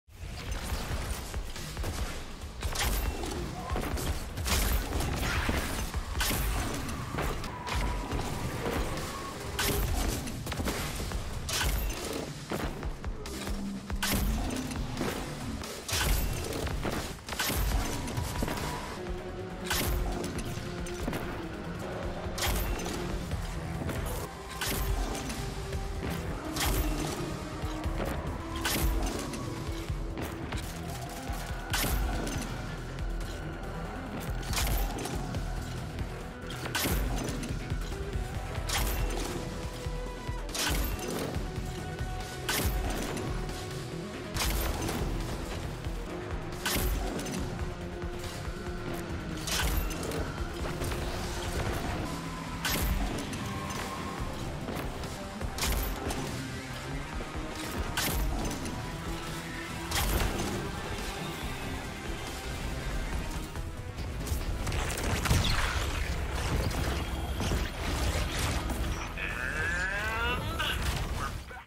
Bugged Fighting Lion vs Parthenios, Drifting Mind in Grandmaster
Honestly a solid choice for this Nightfall with it's high damage against the boss and all red bars. I'm still surprised the weapon has not yet been fixed or even get disabled. The slow reload can be a little annoying, triple void reload can help a bit with the problem.